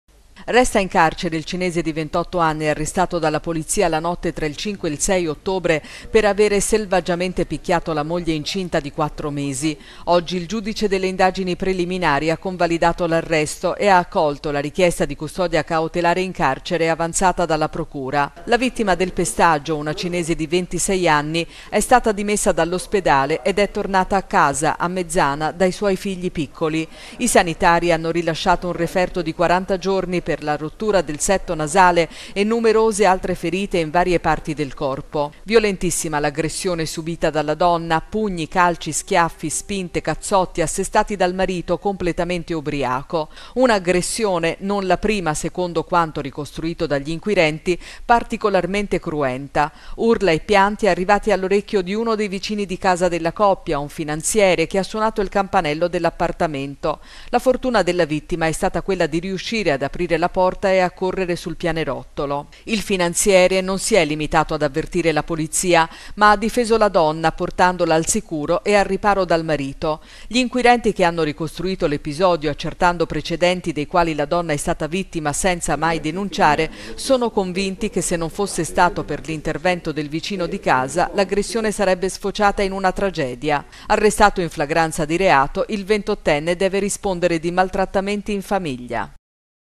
2020-10-09 PRATO - BOTTE A MOGLIE INCINTA, 28ENNE IN CARCERE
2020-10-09 PRATO - BOTTE A MOGLIE INCINTA, 28ENNE IN CARCERE